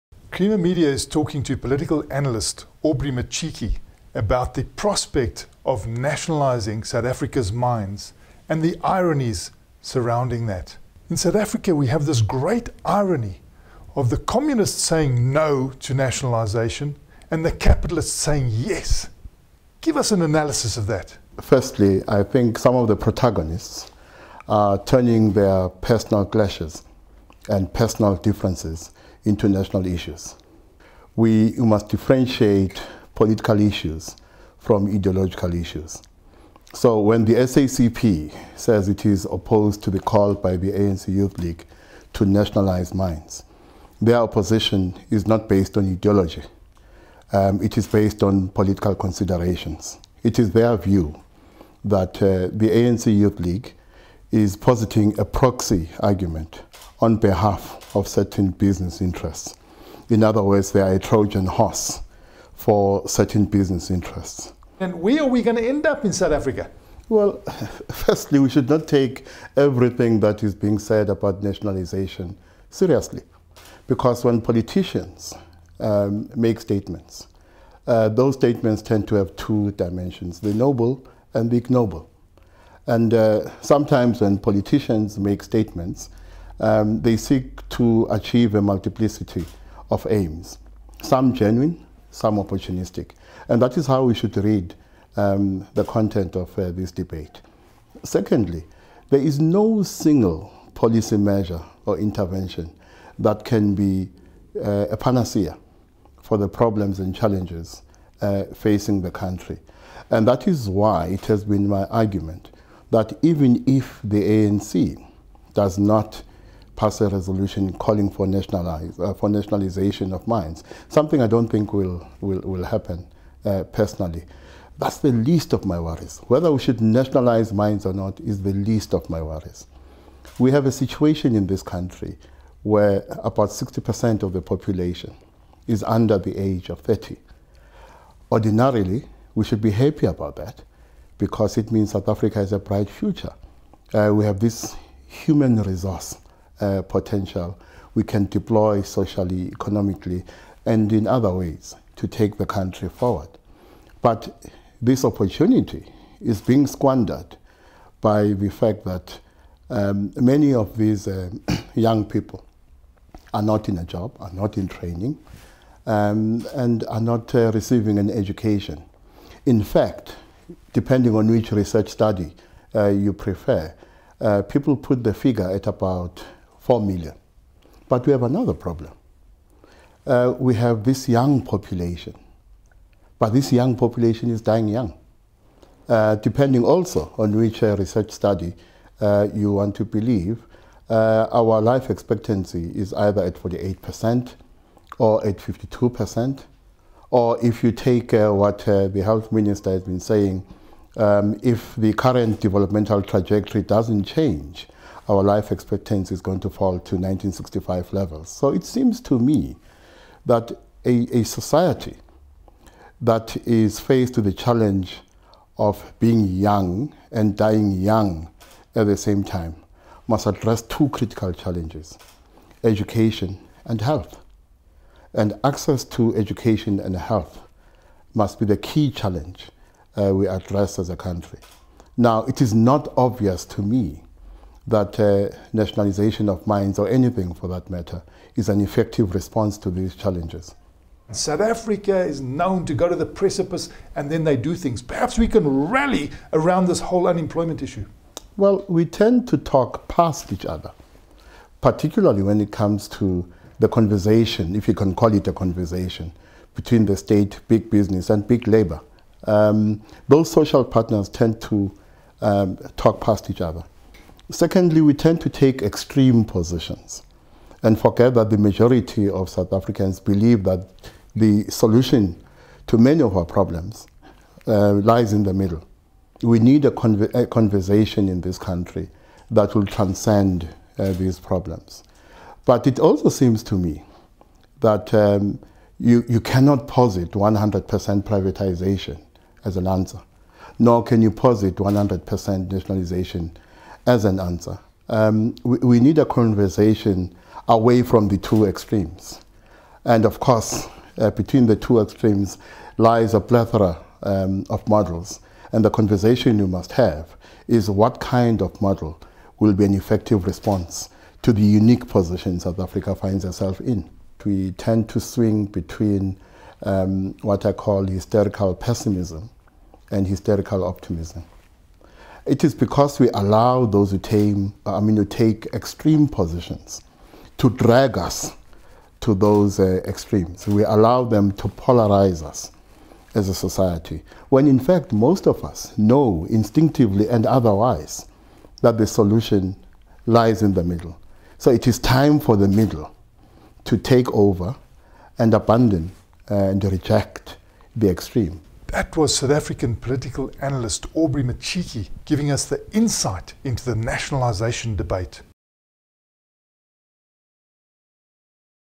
Nationalisation: Centre must reject extremes -- Matshiqi
Helen Suzman Foundation research fellow Aubrey Matshiqi analyses the mine nationalisation debate in conversation with Mining Weekly Online's Martin Creamer.